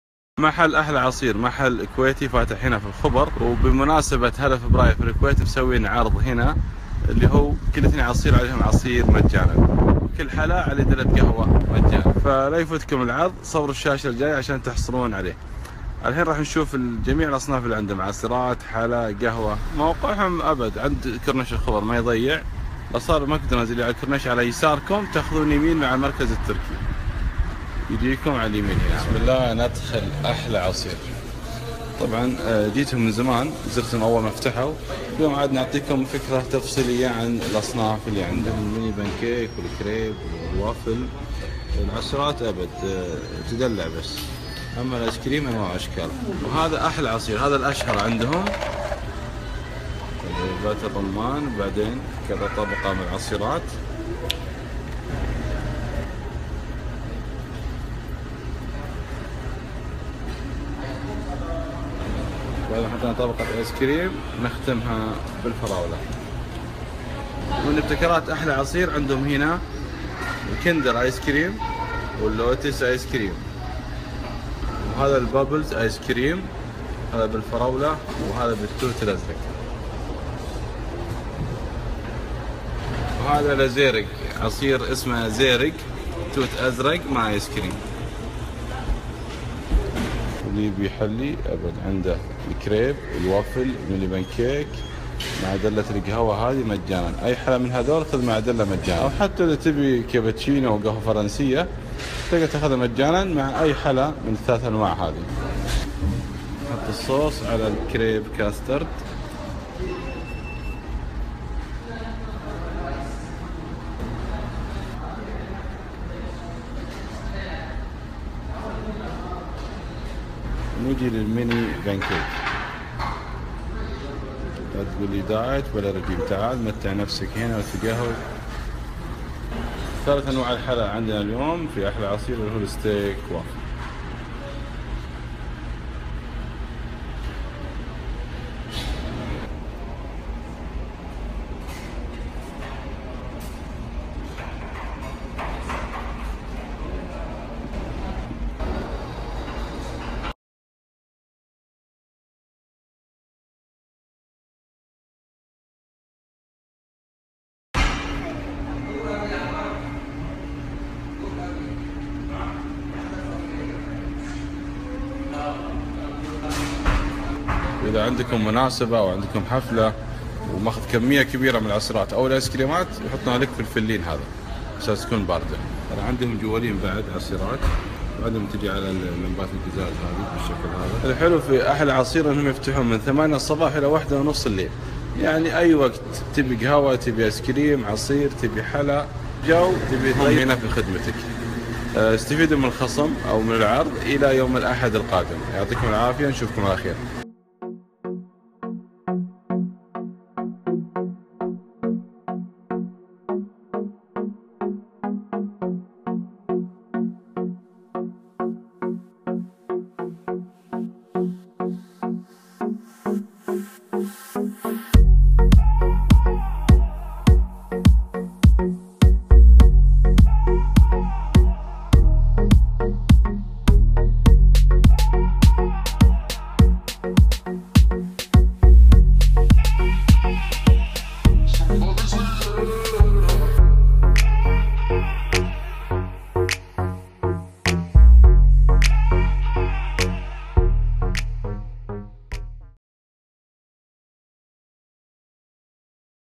لا يفوتكم (احلى عصير) محل العصيرات الكويتي الشهير في الخبر | سناب الشرقية Sharqia1
بمناسبة هلا فبراير في الكويت محل (أحلى عصير) بالخبر مسوين عرض لمتابعي (سناب الشرقية) اشتر ٢ عصير والثالث مجاناً وايضا اشتر اي حلا ودلة القهوة مجاناً،مبدعين بأنواع العصيرات والايسكريم وحلا الكريب والوافل والبانكيك، جربت الكريب كاسترد😍 لا يفوتكم
يفتحون من ٨ الصباح الى ١:٣٠ ليلاً بشكل متواصل، عندهم قسم للعائلات وقسم للافراد، موقعهم كورنيش الخبر خلف مركز التركي وقريباً فرعهم بالدمام حي الشاطئ👏